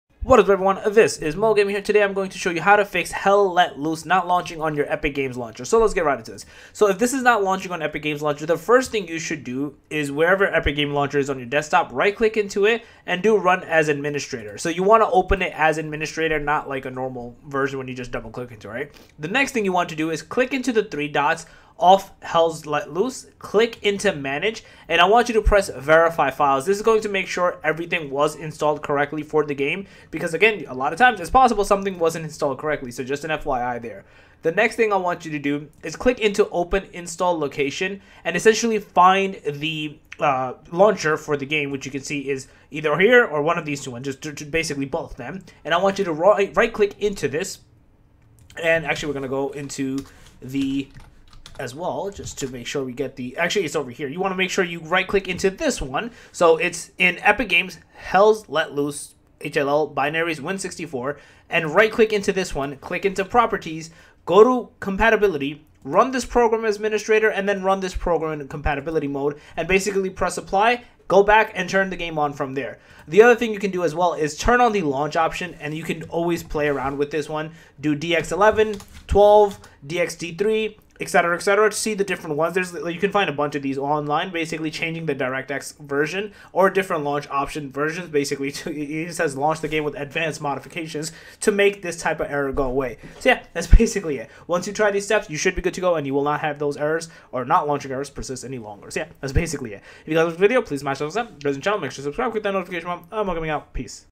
How To Fix Hell Let Loose Not Launching On Epic Games Launcher
In this video, I talk about How To Fix Hell Let Loose Not Launching On Epic Games Launcher